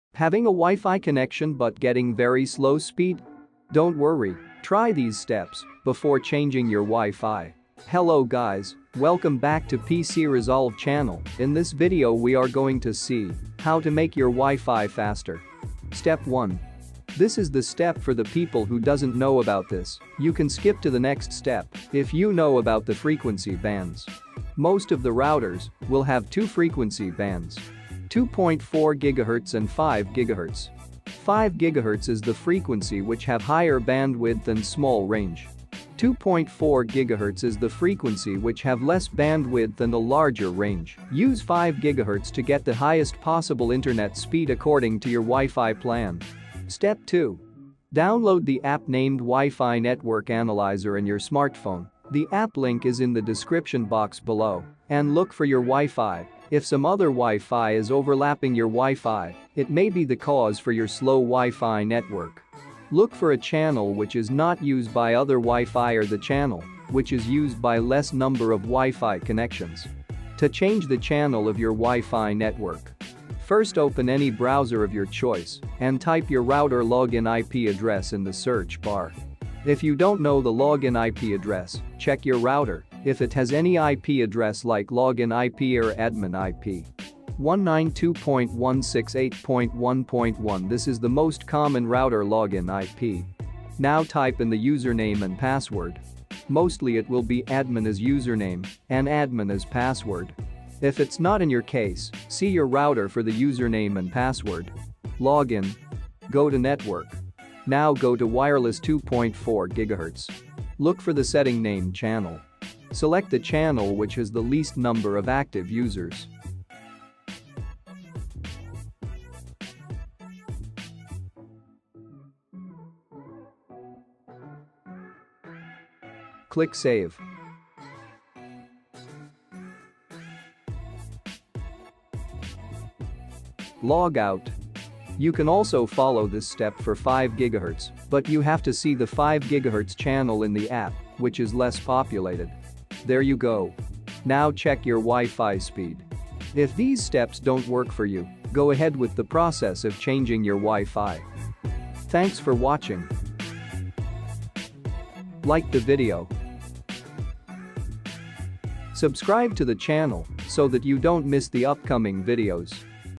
How to increase WiFi speed? | How to increase internet Speed?| 2021| PC Resolve Channel
Hello guys, in this video we are going to see How to increase wifi speed? ?

Timestamps
0:00 - Intro
0:14 - Step 1
0:46 - Step 2
2:31 - Outro